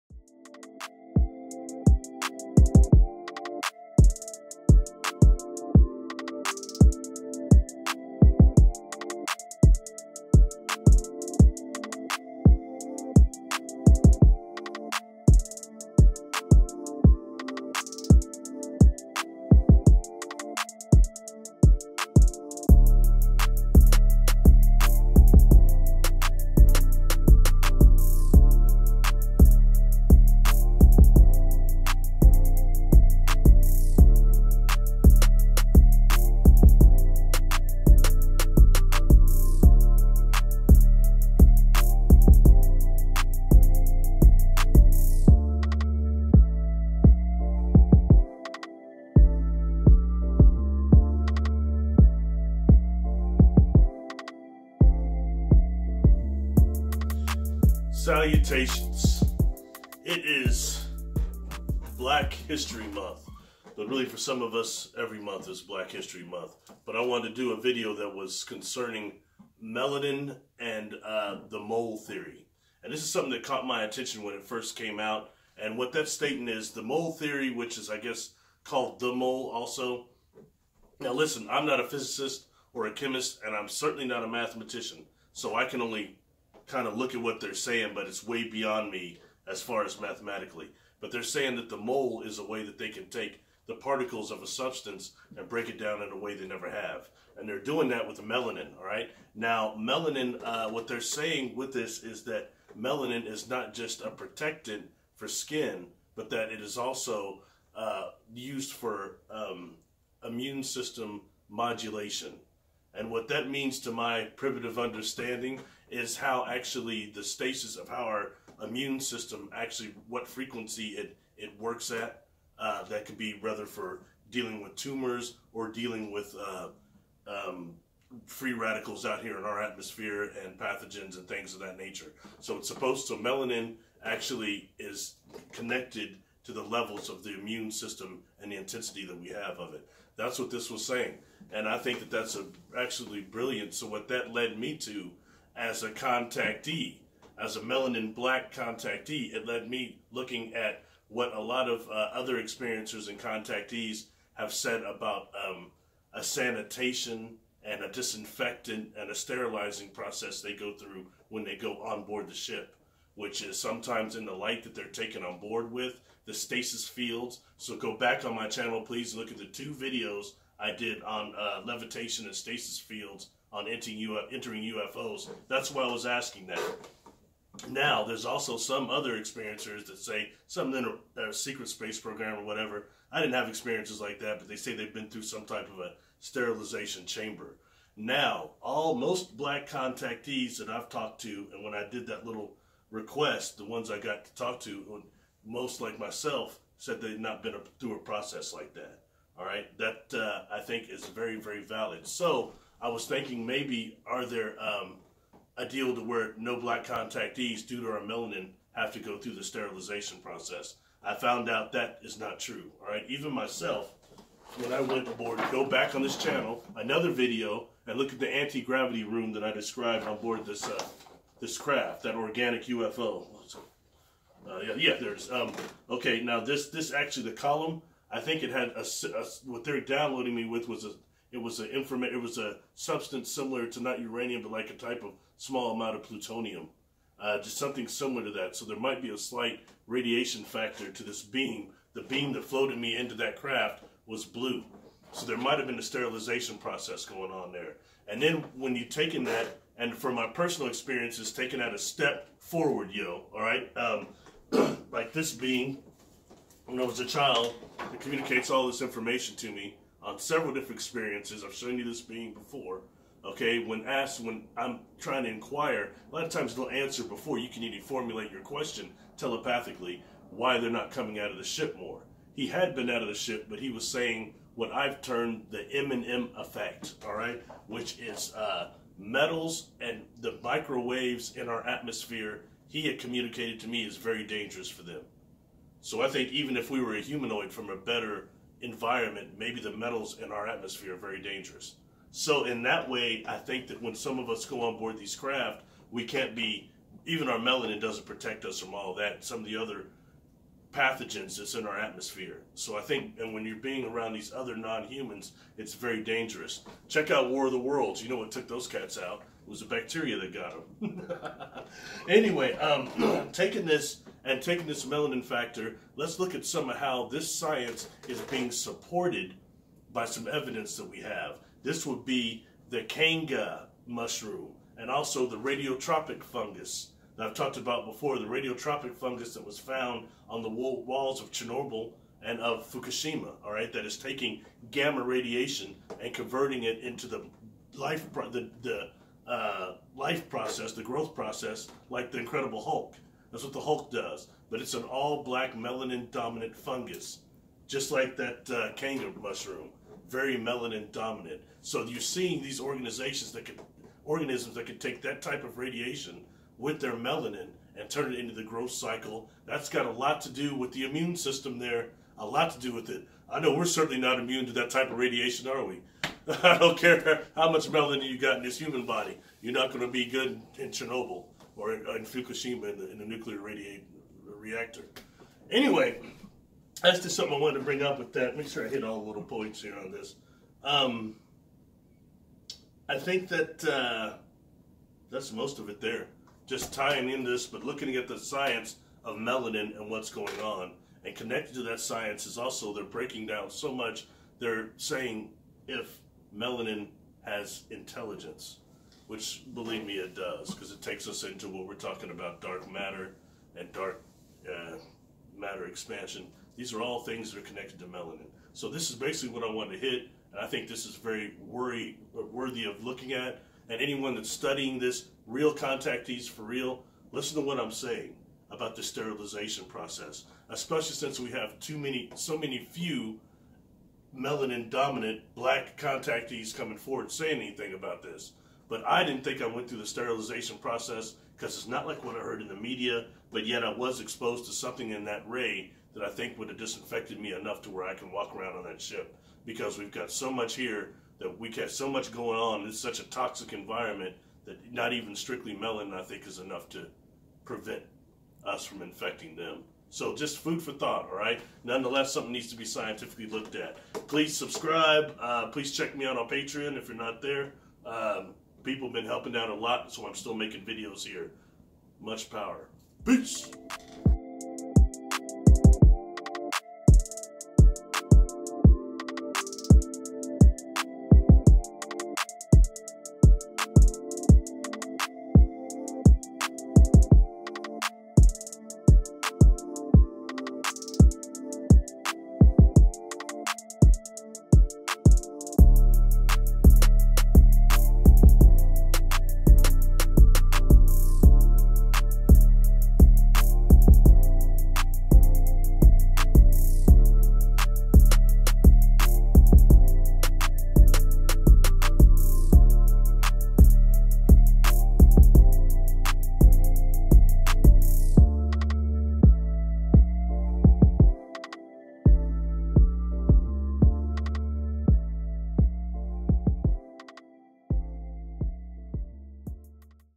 Melanin & Mole Theory..........
Discussing melanin & the immune system. Correlating it to several things, including the Contact Experience.